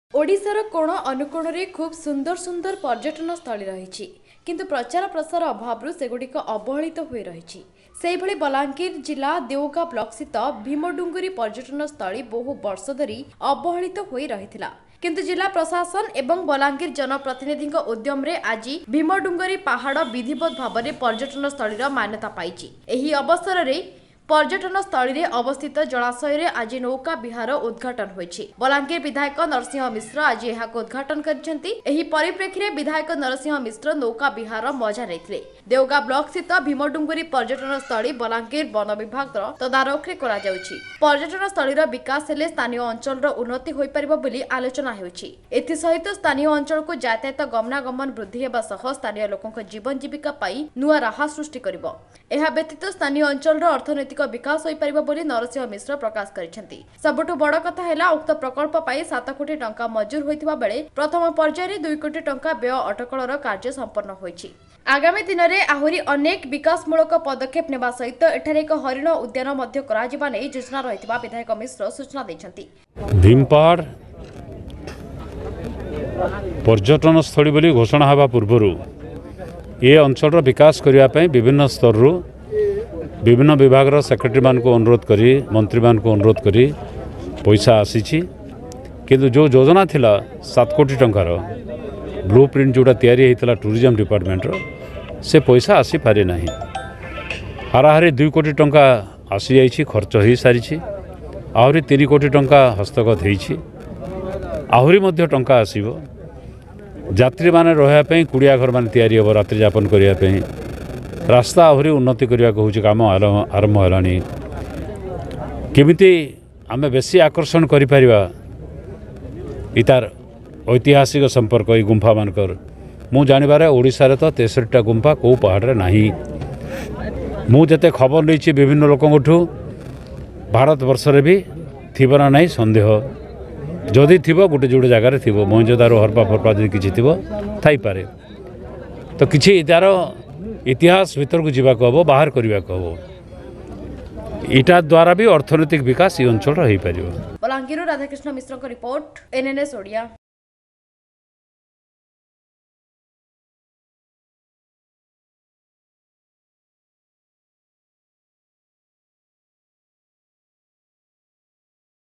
Boat Ride Facilities start in Bhimdungri Tourism place || ପର୍ଯ୍ୟଟନସ୍ଥଳୀର ମାନ୍ୟତା ପାଇଲା ଭୀମଡୁଙ୍ଗୁରି
ଓଡ଼ିଶାର କୋଣ ଅନୁକୋଣରେ ଖୁବ ସୁନ୍ଦର ସୁନ୍ଦର ପର୍ଯ୍ୟଟନସ୍ଥଳୀ ରହିଛି | କିନ୍ତୁ ପ୍ରଚାର ପ୍ରସାର ଅଭାବରୁ ସେଗୁଡିକ ଅବହେଳିତ ହୋଇରହିଛି | ସେହିଭଳି  ବଲାଙ୍ଗୀର ଜ଼ୀଲ୍ଲା ଦେଓଗାଁ ବ୍ଲକ ସ୍ଥିତ ଭୀମଡୁଙ୍ଗୁରି ପର୍ଯ୍ୟଟନ ସ୍ଥଳୀ ବହୁ ବର୍ଷ ଧରି ଅବହେଳିତ ହୋଇ ରହିଥିଲା | କିନ୍ତୁ ଜିଲ୍ଲା ପ୍ରଶାସନ ଏବଂ ବଲାଙ୍ଗୀର ଜନ ପ୍ରତିନିଧିଙ୍କ ଉଦ୍ୟମରେ ଆଜି ଭୀମଡୁଙ୍ଗରି ପାହାଡ଼ ବିଧିବଦ୍ଧ ଭାବରେ ପର୍ଯ୍ୟଟନସ୍ଥଳୀର ମାନ୍ୟତା ପାଇଛି | ଏହି ଅବସରେ ପର୍ଯ୍ୟଟ ସ୍ଥଳୀରେ ଅବସ୍ଥିତ ଜଳାଶୟରେ ଆଜି ନୌକା ବିହାରର ଉଦଘାଟନ ହୋଇଛି । ବଲାଙ୍ଗୀର ବିଧାୟକ ନରସିଂହ ମିଶ୍ର ଆଜି ଏହାକୁ  ଉଦ୍ଘାଟନ କରିଛନ୍ତି । ଏହି ପରିପେକ୍ଷୀରେ ବିଧାୟକ ନରସିଂହ ମିଶ୍ର ନୌକା ବିହାରର ମଜା ନେଇଥିଲେ |

ଦେଓଗାଁ ବ୍ଲକ ସ୍ଥିତ ଭୀମଡୁଙ୍ଗୁରି ପର୍ଯ୍ୟଟନସ୍ଥଳୀ  ବଲାଙ୍ଗୀର ବନ ବିଭାଗର ତଦାରଖରେ କରାଯାଉଛି | ପର୍ଯ୍ୟଟନସ୍ଥଳୀର ବିକାଶ ହେଲେ  ସ୍ଥାନୀୟ ଅଞ୍ଚଳର  ଉର୍ନତି ହୋଇପାରିବ ବୋଲି ଆଲୋଚନା ହେଉଛି | ଏଥିସହିତ ସ୍ଥାନୀୟ ଅଞ୍ଚଳକୁ ଯାତାୟତ, ଗମନାଗମନ ବୃଦ୍ଧି ହେବା  ସହ  ସ୍ଥାନୀୟ ଲୋକଙ୍କ   ଜୀବନ ଜୀବିକା ପାଇଁ ନୂଆ ରାହା ସୃଷ୍ଟି କରିବ  । ଏହାବ୍ୟତୀତ ସ୍ଥାନୀୟ ଅଞ୍ଚଳର ଅର୍ଥନୈତିକ ବିକାଶ ହୋଇପାରିବ ବୋଲି ନରସିଂହ ମିଶ୍ର  ପ୍ରକାଶ କରିଛନ୍ତି । ସବୁଠୁ ବଡ଼ କଥା ହେଲା ଉକ୍ତ ପ୍ରକଳ୍ପ ପାଇଁ  7 କୋଟି ଟଙ୍କା ମଞ୍ଜୁର ହୋଇଥିବା ବେଳେ ପ୍ରଥମ ପର୍ଯ୍ୟାୟରେ 2କୋଟି ଟଙ୍କା ବ୍ୟୟ ଅଟକଳର କାର୍ଯ୍ୟ ସମ୍ପର୍ଣ୍ଣ ହୋଇଛି ।   ଆଗାମୀ ଦିନରେ ଆହୁରୀ ଅନେକ ବିକାଶମୁଳକ ପଦକ୍ଷେପ ନେବା ସହିତ ଏଠାରେ ଏକ ହରିଣ ଉଦ୍ୟାନ ମଧ୍ୟ କରାଯିବା ନେଇ ଯୋଜନା ରହିଥିବା ବିଧାୟକ ମିଶ୍ର ସୂଚନା ଦେଇଛନ୍ତି ।